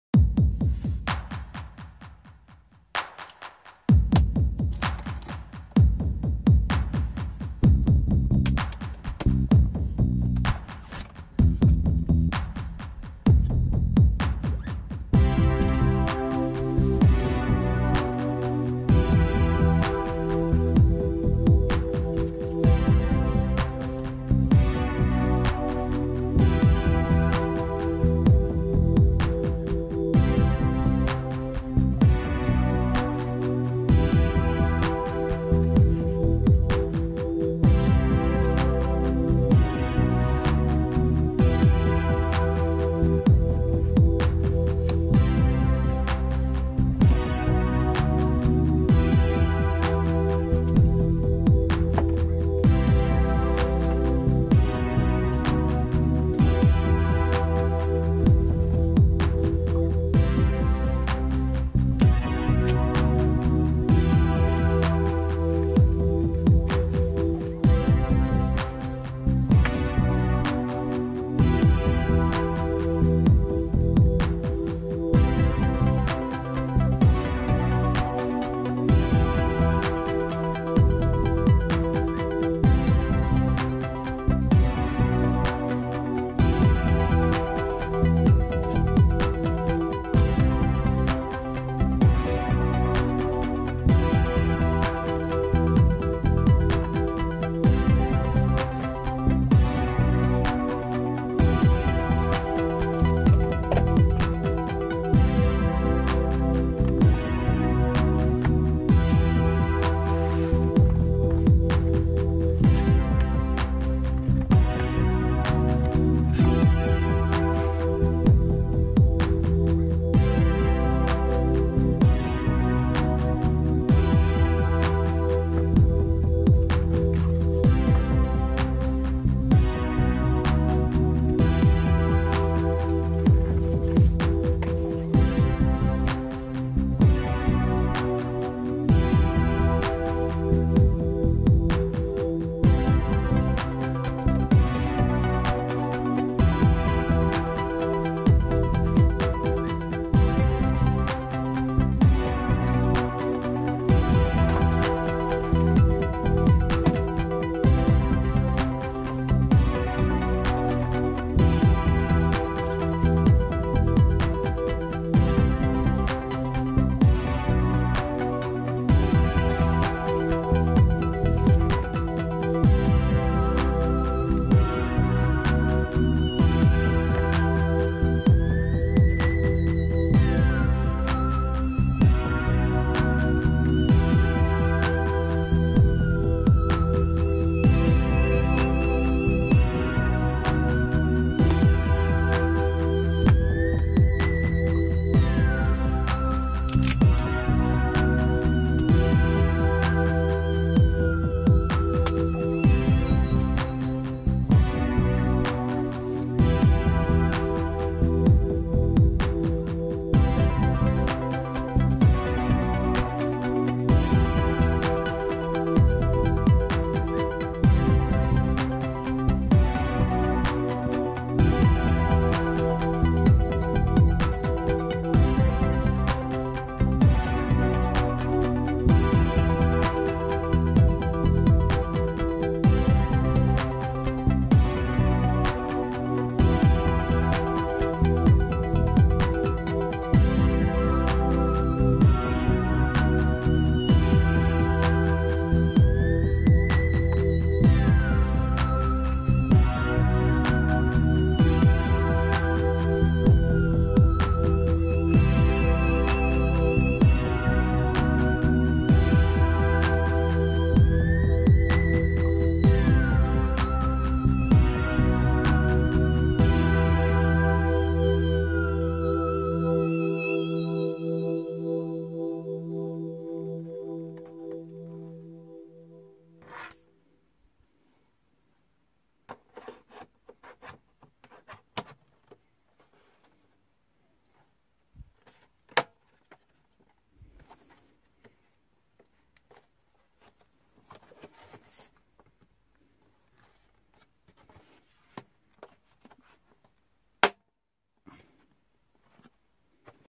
Xbox one S Unboxing
Your boi Distinguished opening the new xbox One S